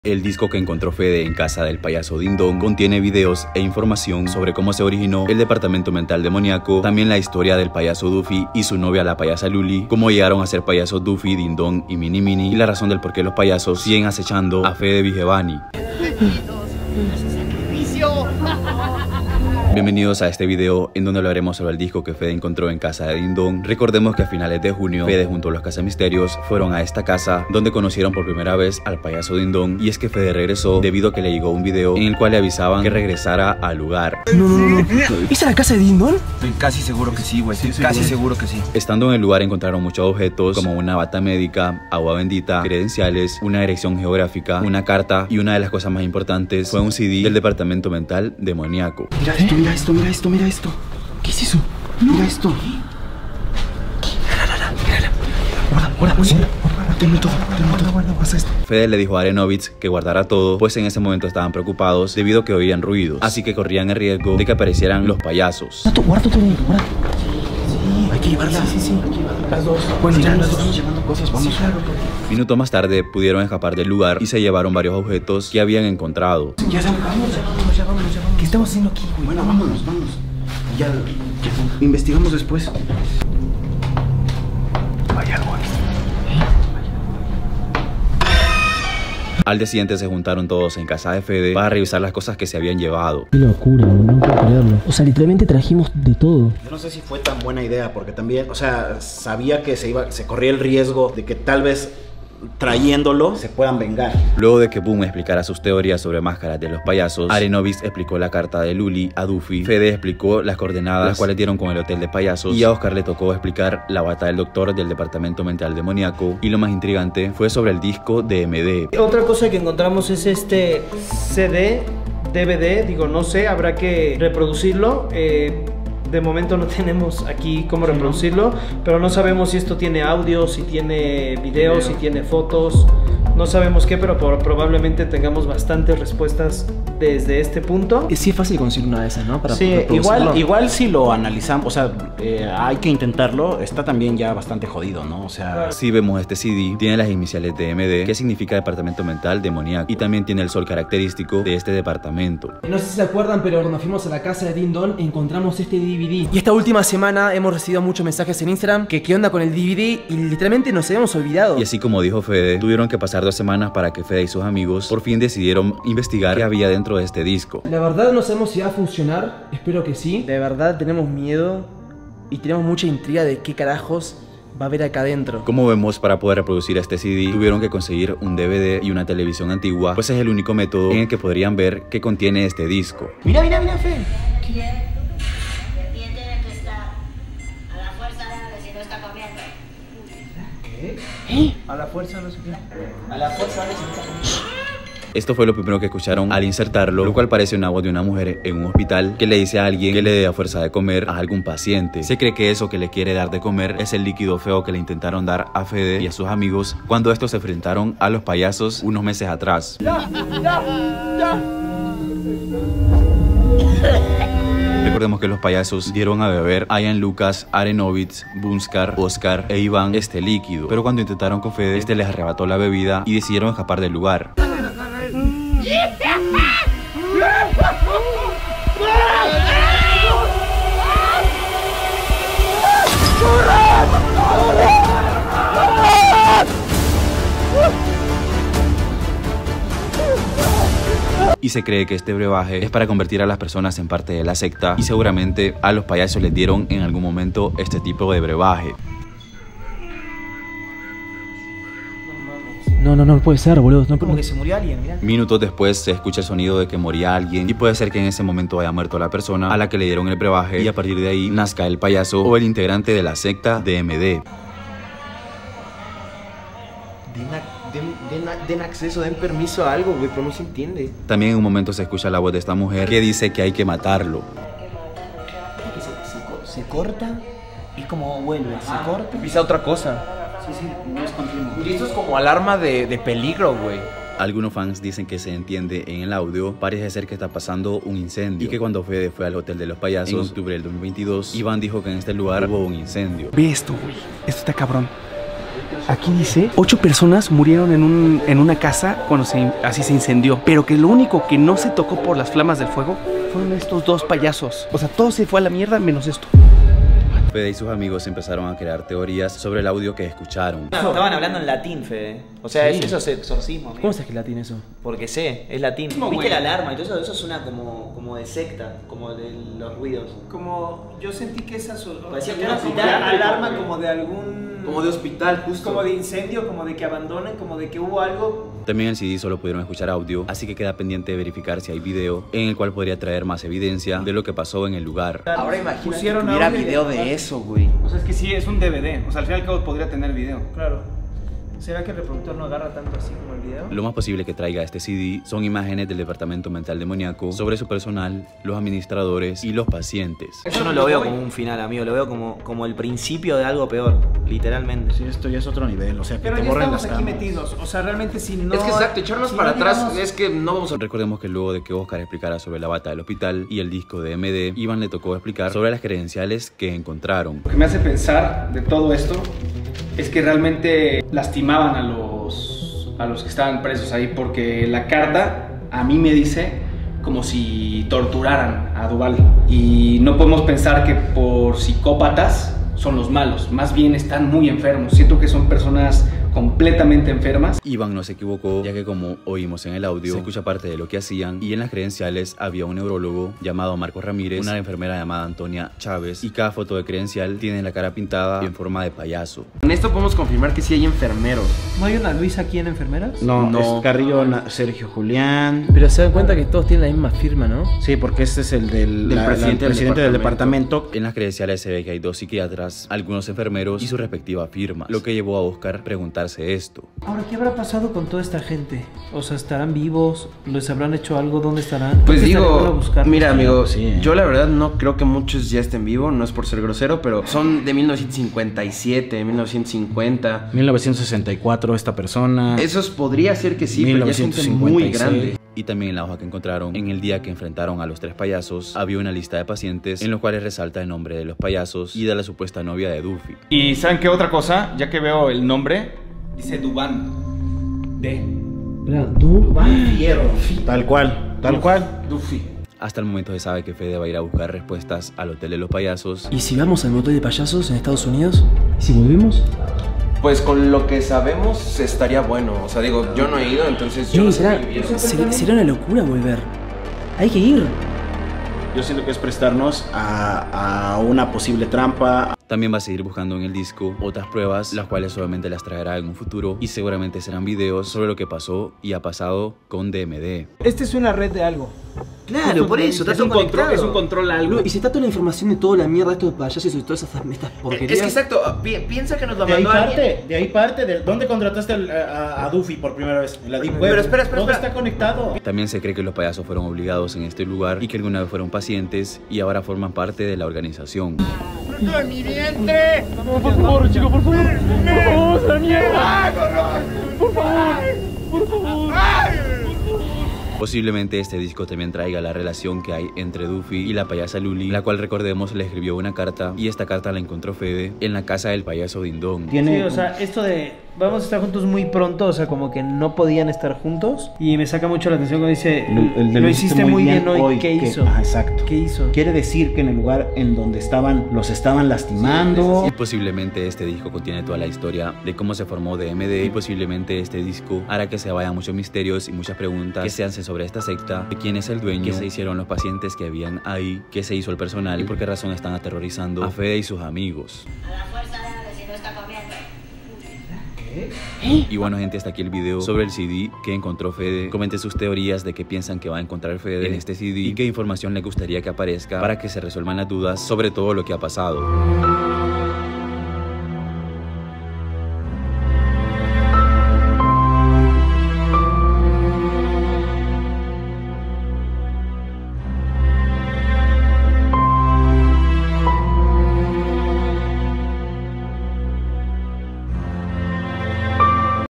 ESTO encontré en el Disco DMD de LOS PAYASOS DE LA DEEP WEB 💿😱
Hola chicos bienvenidos a este video donde las traigo teorías sobre el Disco CD que encontró @fedevigevani  en casa del payaso Din Don, y Que pusieron en su televisor en donde hay audios difíciles de entender y lo que posiblemente tendrá en un futuro sobre el Departamento Mental Demoníaco

Recordando que El Payaso Duffy, el payaso mini mini y el payaso din don son de la deep web, y están en casa de Fede Vigevani